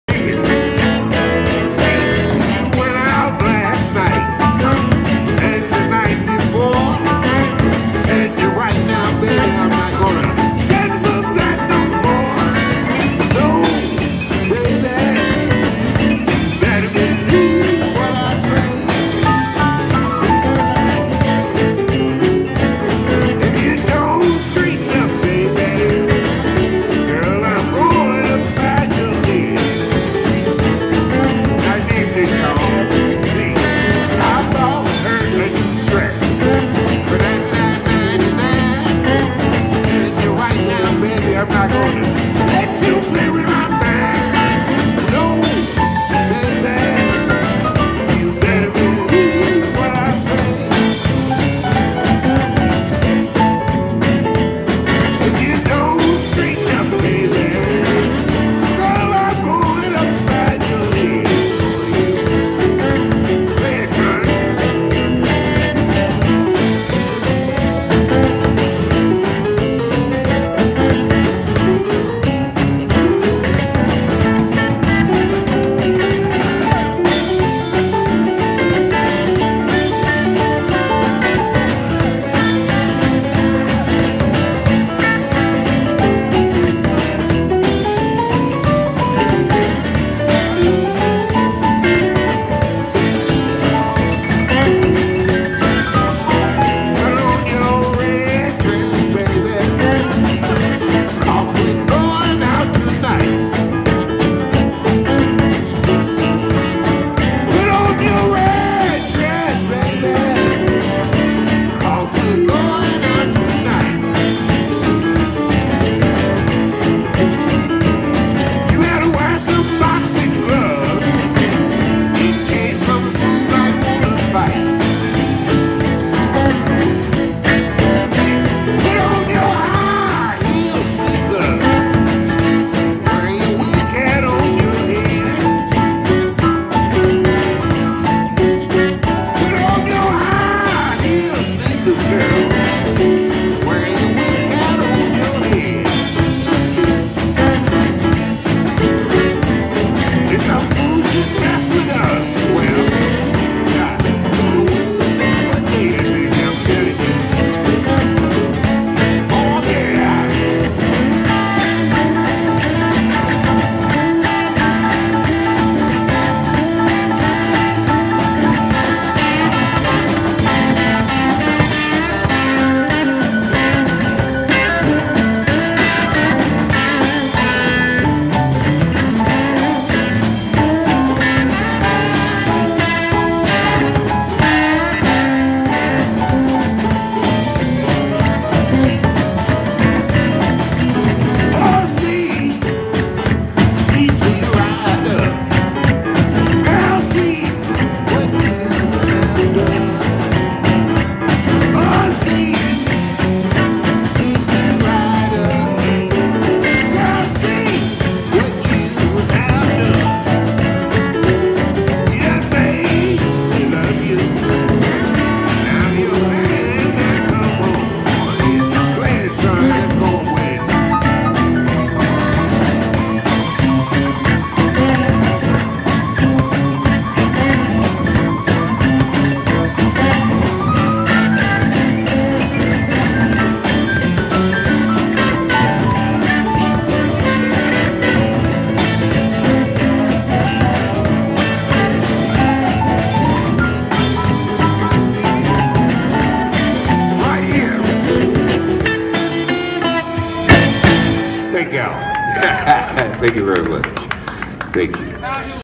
Blues Trail Celebration
Bill "Howl-N-Madd" Perry performs as part of the celebration of the Mississippi Blues Trail at the Tanger Outlets in Southaven, MS.